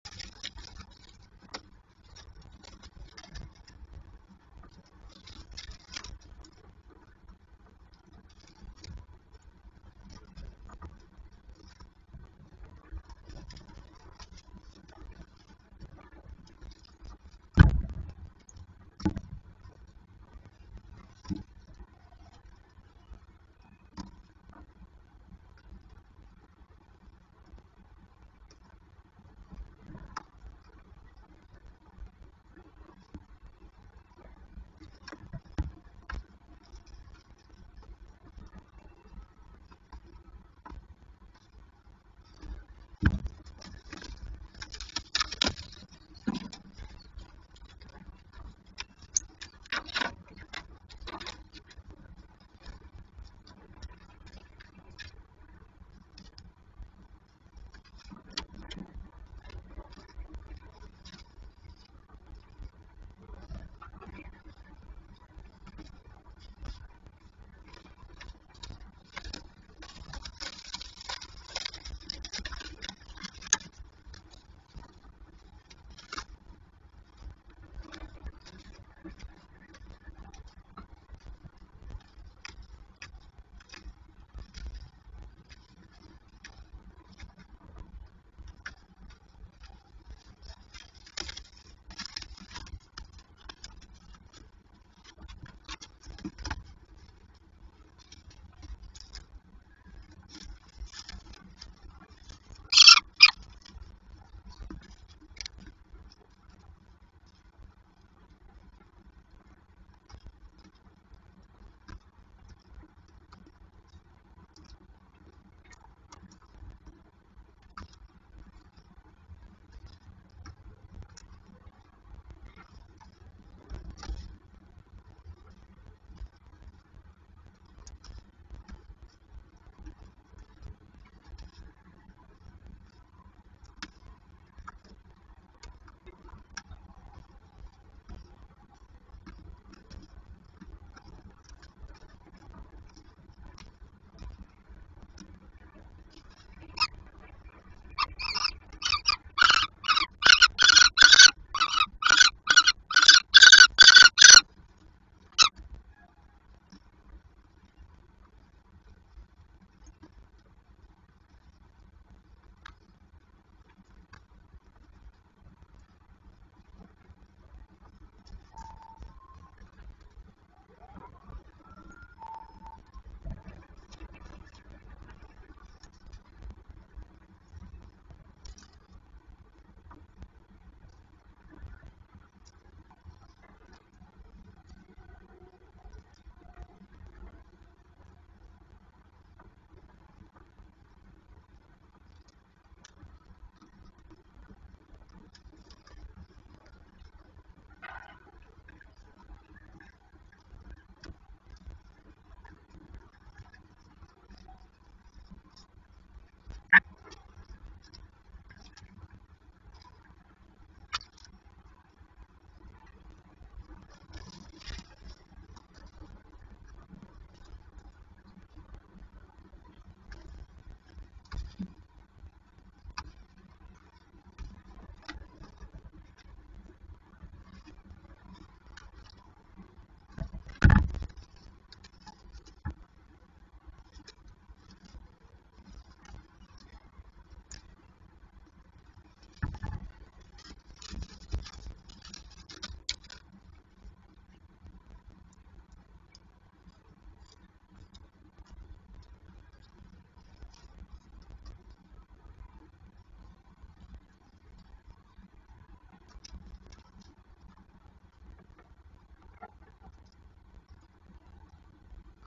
PGE Lublin Poland -Bystry joined to girls, calmly eat the bird in three - 2019 05 27 13 42
2 BM BYSTRY; 3 BM BYSTRA;  4 BM LUBELKA